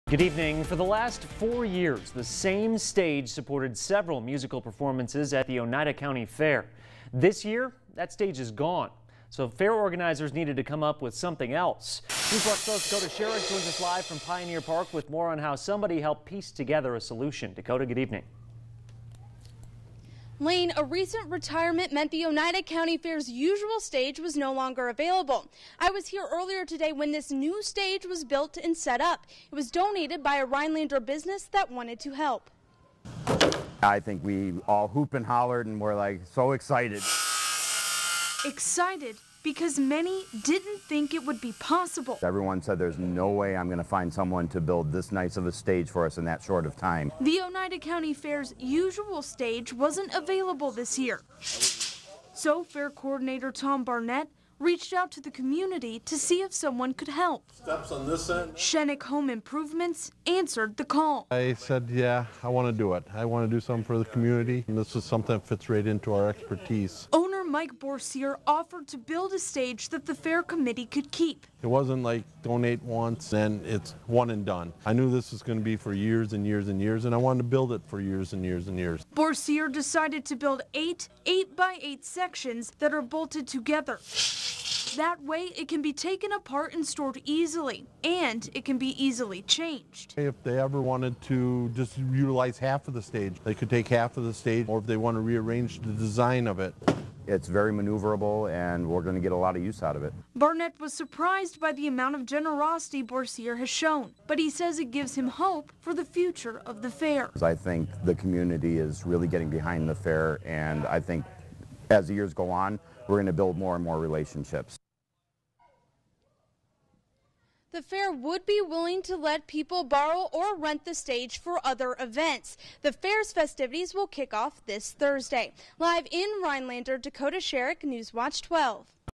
Rhinelander business donates new stage to Oneida County Fair
The Oneida County Fair's usual stage wasn't available this year. So fair coordinator Tom Barnett reached out to the community to see if someone could help. Schoeneck Home Improvements answered the call.